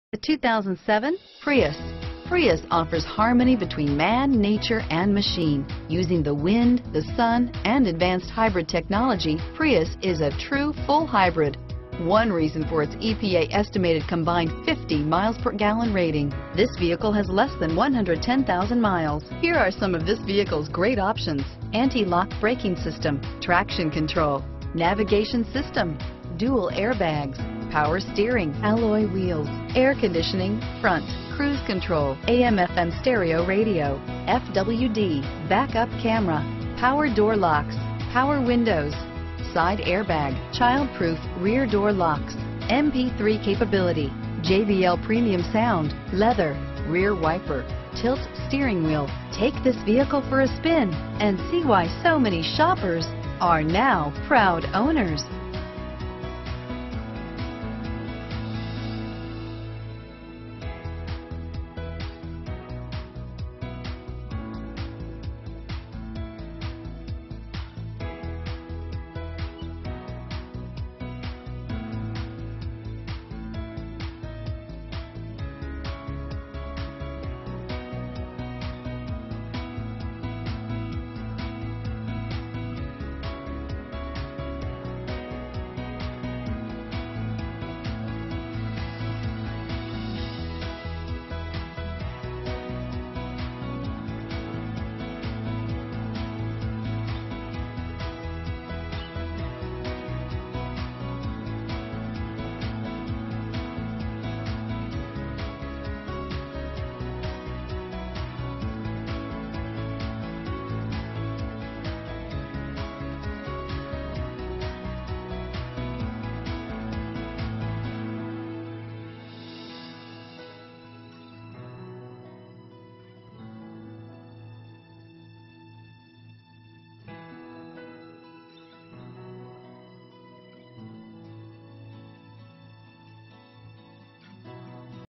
2007 Toyota Prius - Touring Hatchback 4D Phoenix AZ 620810
2007 Toyota Prius  - Stock#: 620810

1.5L I4 SMPI DOHC, ABS brakes, Alloy wheels, Heated door mirrors, Low tire pressure warning, PACKAGE 6 LEATHER NAVIGATION AND MORE, Remote keyless entry, and Traction control. Don't pay too much for the car you want...Come on down and take a look at this hardy, reliable 2007 Toyota Prius. The Hybrid engine not only has plenty of power, but also still manages to give you outstanding fuel economy. It scored the top rating in the IIHS frontal offset test.